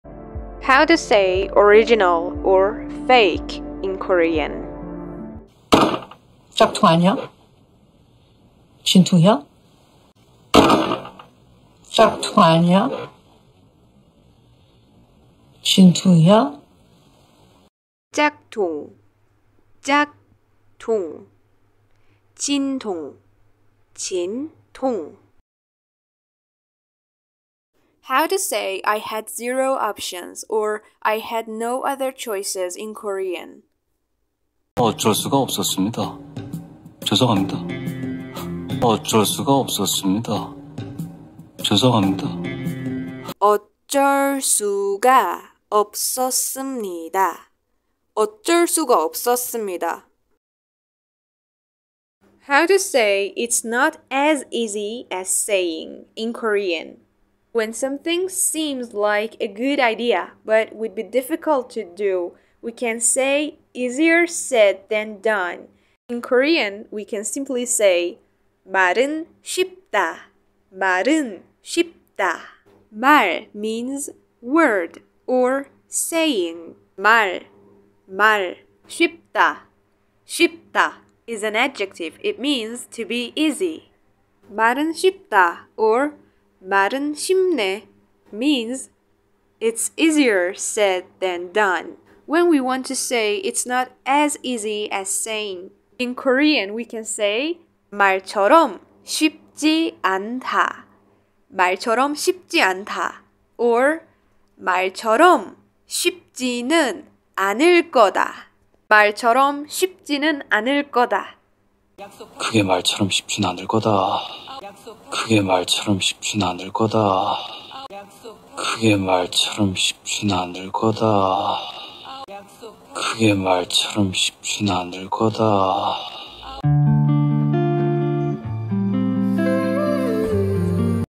learn 5 Korean idioms in 2 minutes! easy Korean
زبان کره ای رو راحت بهت آموزش میدم!
 !learn Korean easily and fun with me
learn with Kdrama